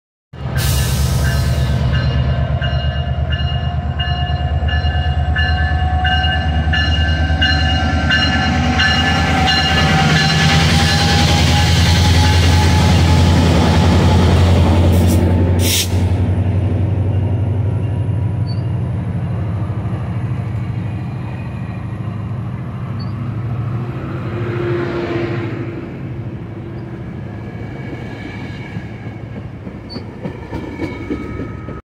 EB Metrolink Train At Commerce Ca!
This eastbound Metrolink train went by here at Commerce Ca!

Date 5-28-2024 Time 4:05pm M686 LA Union Station, CA-Irvine, CA

San Bernardino Sub Milepost 148.9 Mainline 3 Lead Engine
901-MP36PH-3C (MPI) (Metrolink) (Leading Unit)